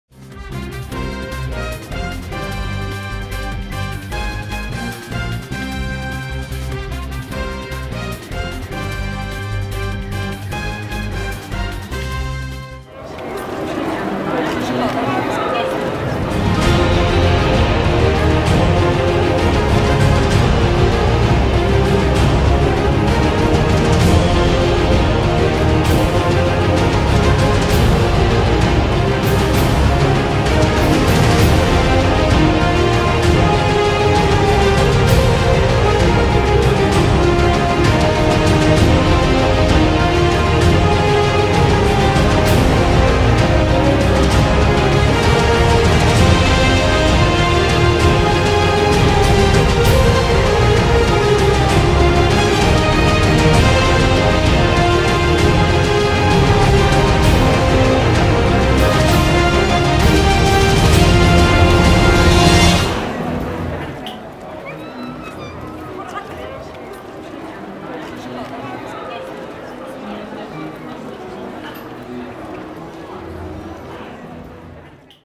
Carnaval Sztuk - Mistrzów - trailer. Lublin 2014 - TOtv.pl
Nasza kolejna realizacja - cykliczna już lubelska impreza z międzynarodowym gronem - Carnaval Sztuk - Mistrzów.